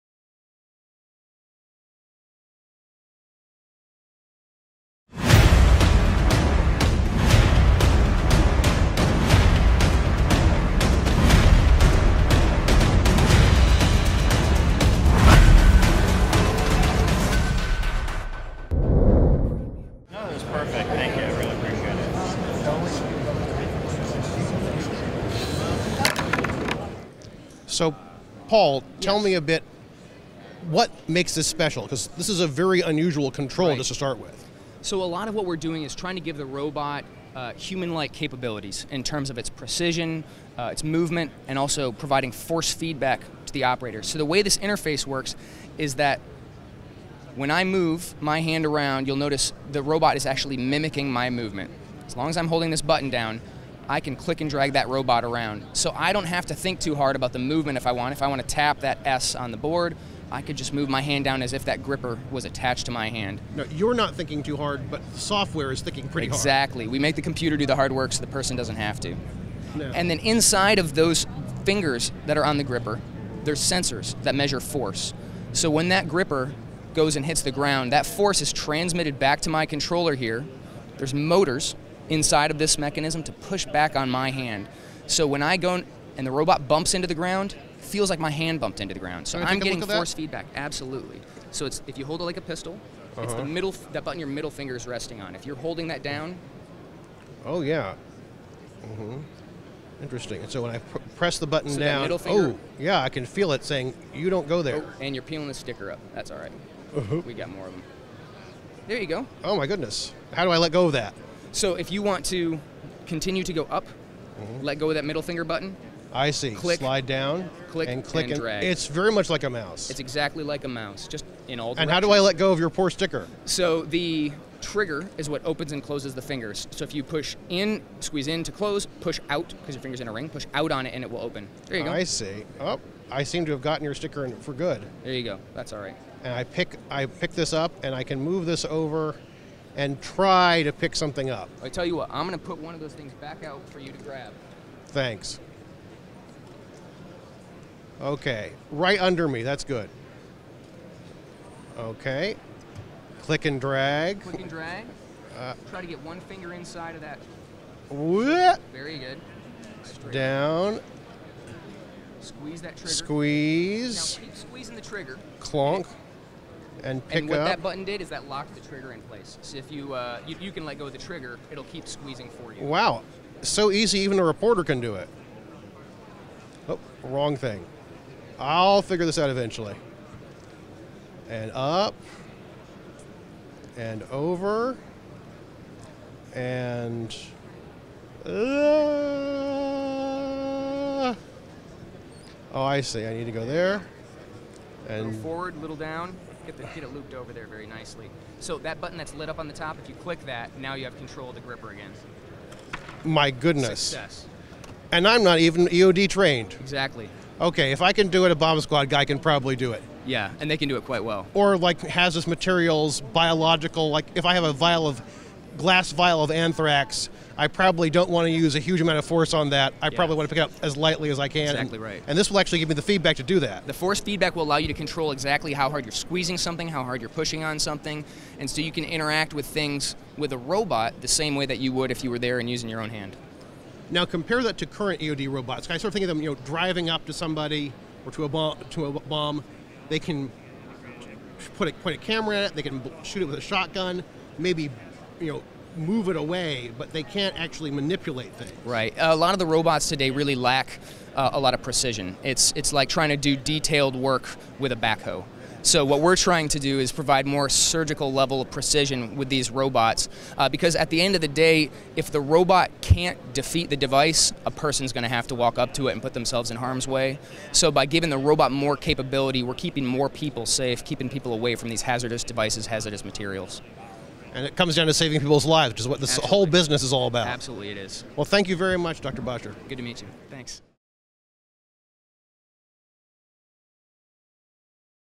AUSA 2016 - Harris Bomb-Squad Bot Has A Delicate Touch
Cut the red wire? Cut the blue wire? That classic action movie dilemma isn’t even on the table for bomb squads using current robots, which can drag the bomb away from people or blast it with a shotgun. That’s what Harris has developed its T7 Explosive Ordnance Disposal robot, which has fingers — sort of.